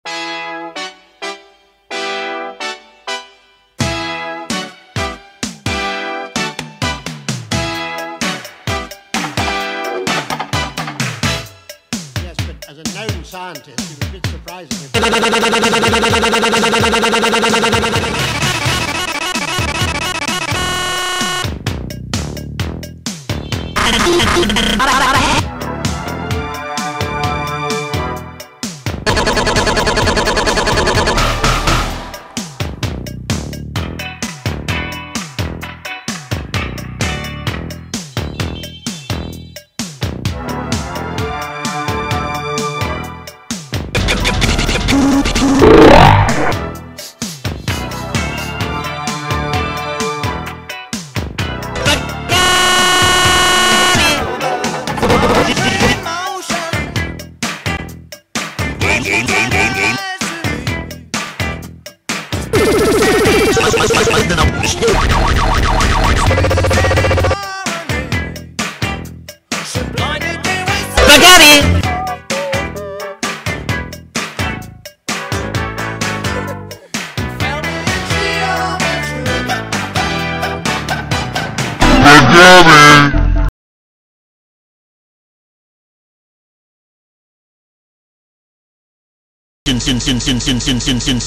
Mario Blinds Luigi with Spaghetti
Attempting to reupload a video i made in 2011 but will probably get autoblocked.
I forget why i have this one in particular saved, i think it was also getting blocked a lot at the time so i put it in dropbox or something